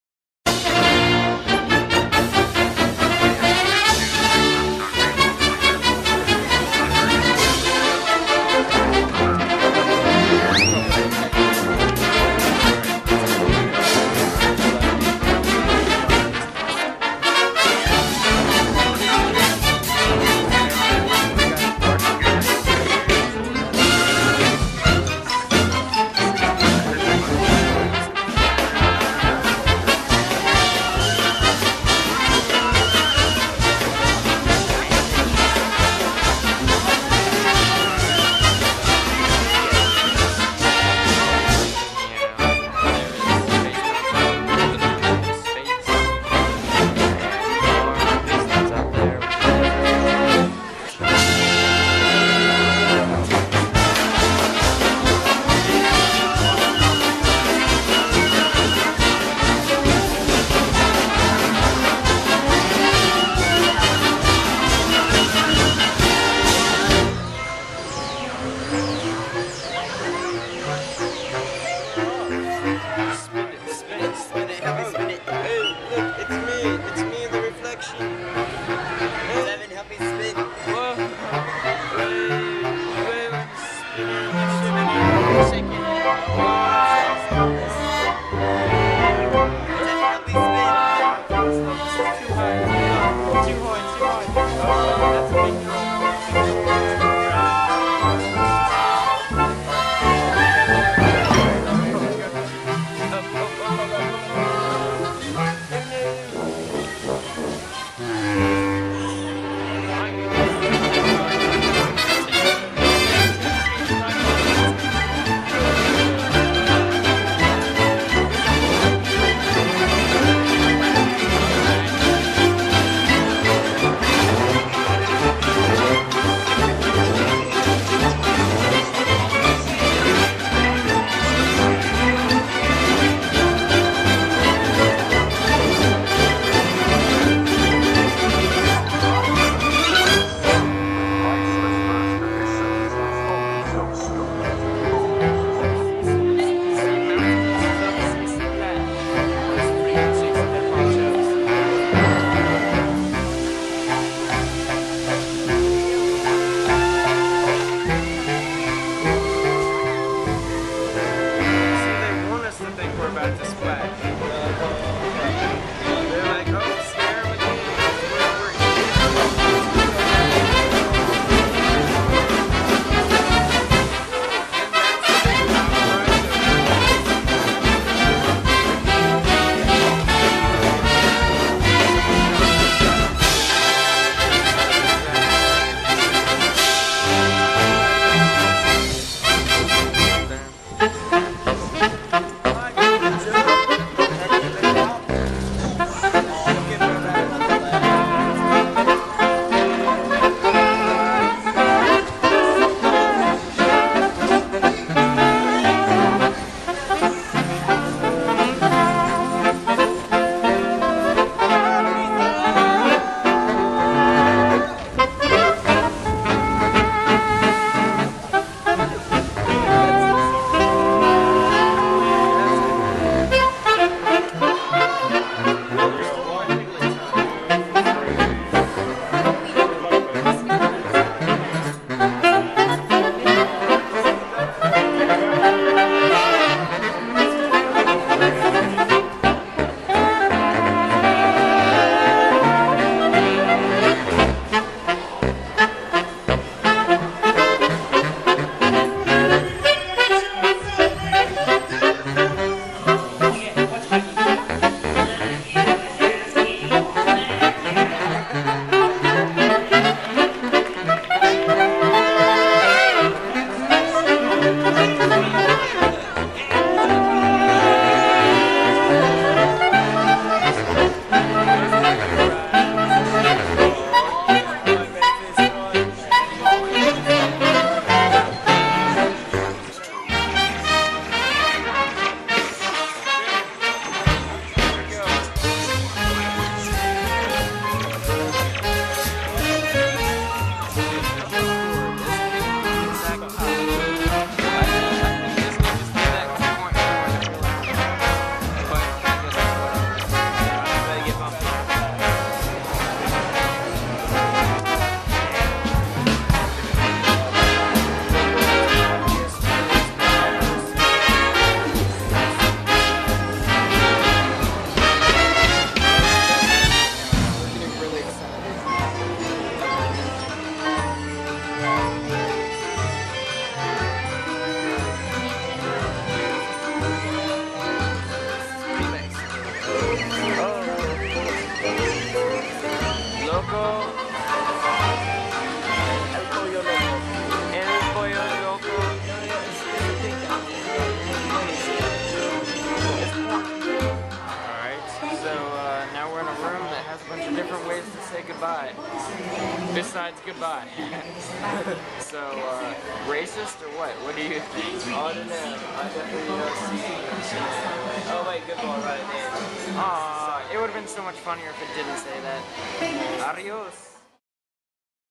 Walt Disney World trip 2012
Highlights from my trip to Disney World with the Skinner family!